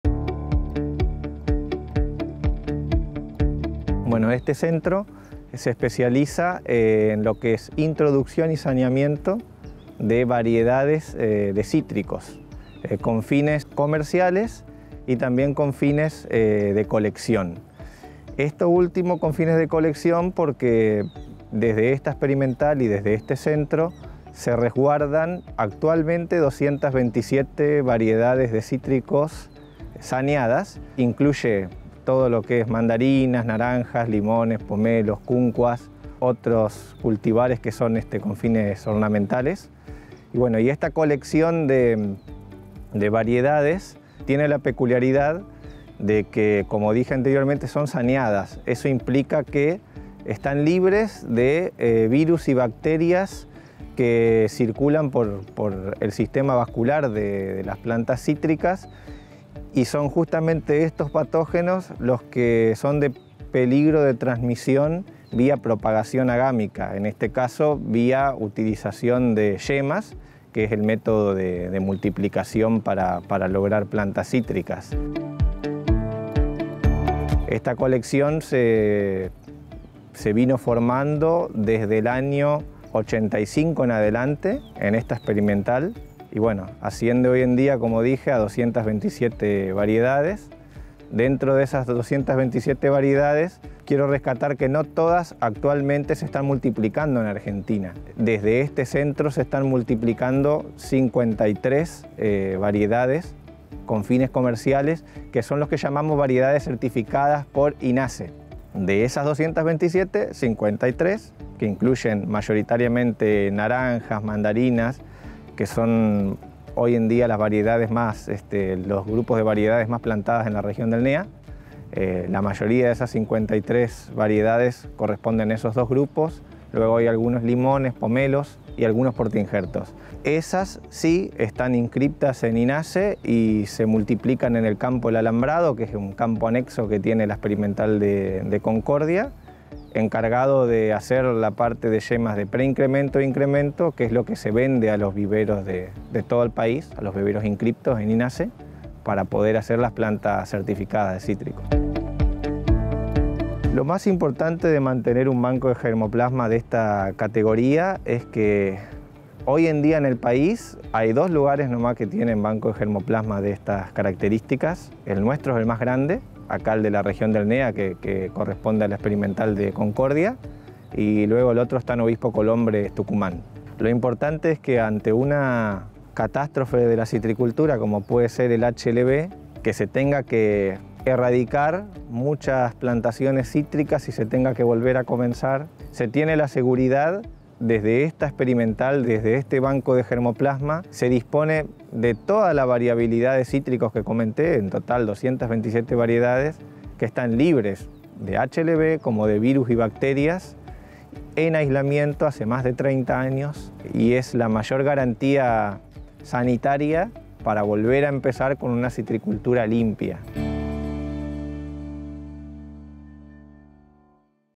Resguardo para la citricultura
Claudio Gómez, responsable del Centro Único de Introducción y Saneamiento, nos cuenta sobre el Banco de Germoplasma de Variedades Cítricas Comerciales Saneadas del INTA Concordia, el más grande del país.